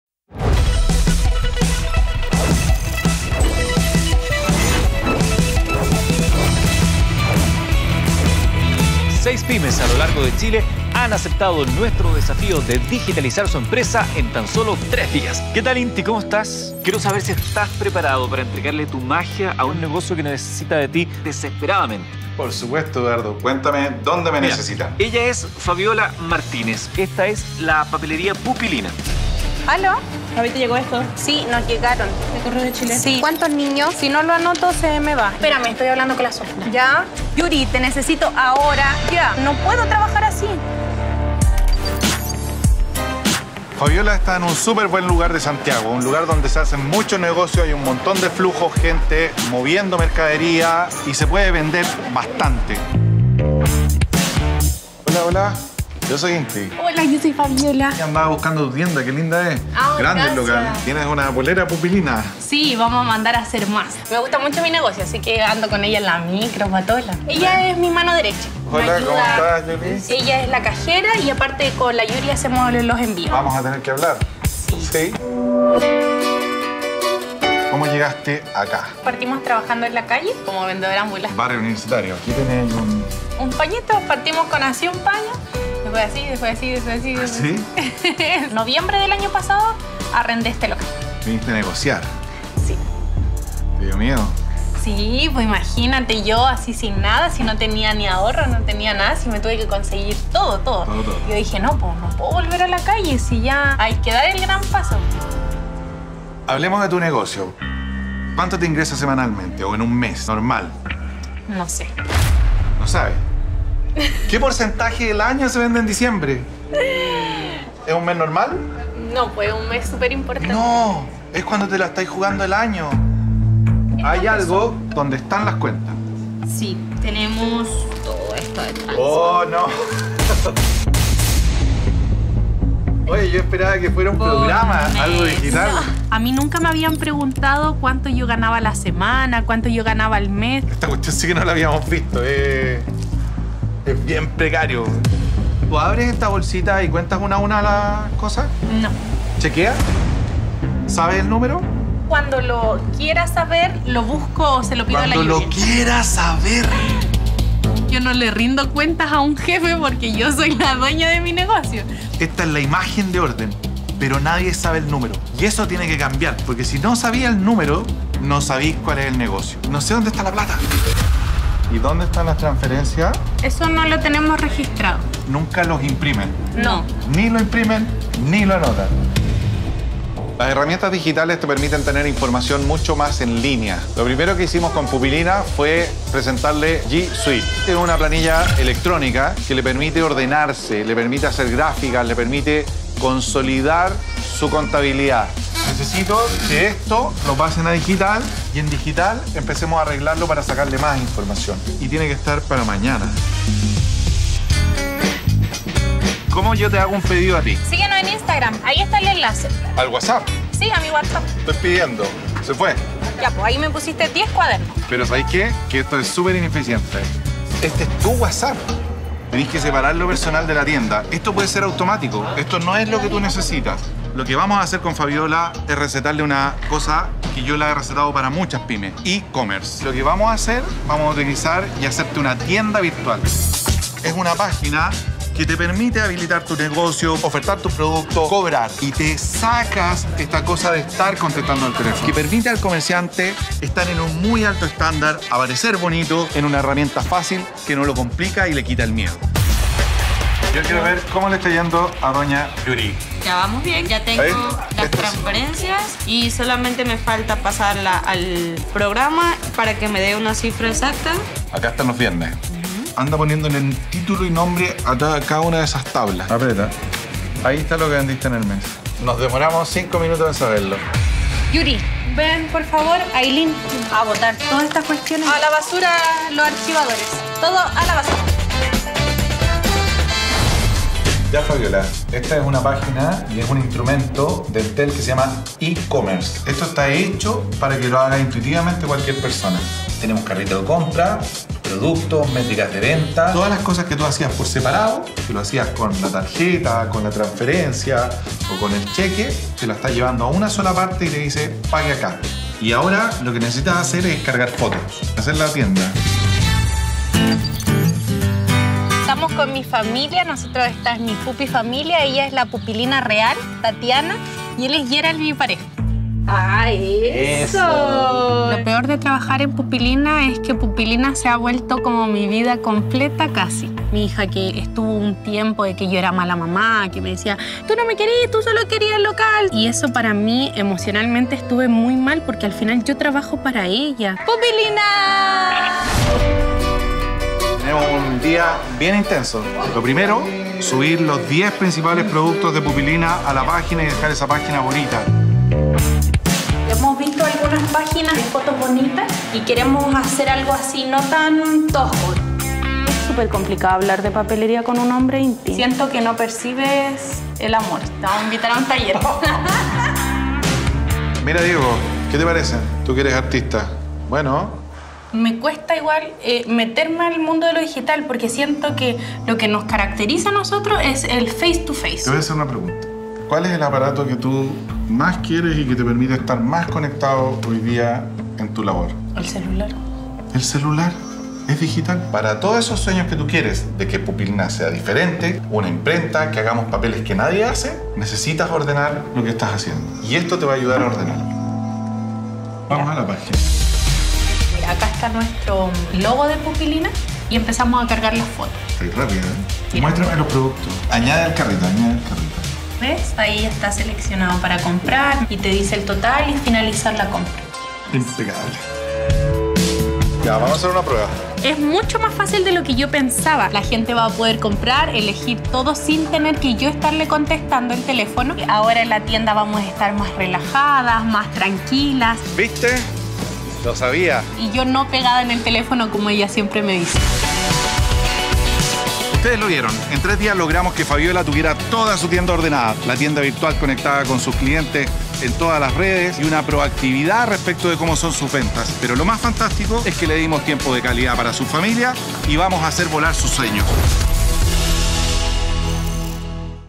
Digitalizados 2 | Entendiendo el negocio
Con la ayuda del coach Inti Núñez y de la digitalización, verá cómo su papelería al fin podrá conseguir el éxito que por mucho tiempo buscó en otros lugares.